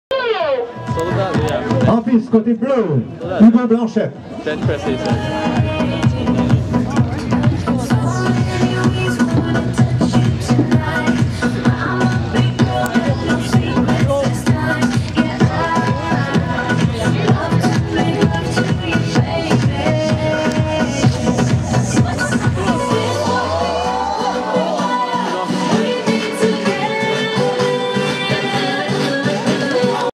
Hugo Blanchette - 2014 FIS Nor-AM - Val St Come - Mens Qualification
The Mens Qualification run by Hugo Blanchette on Day 1 of the 2014 FIS Freestyle North American Cup Mogul Competition at Val St Come on Alexandre Bilodeau. for more videos of mogul skiing from this event and other events.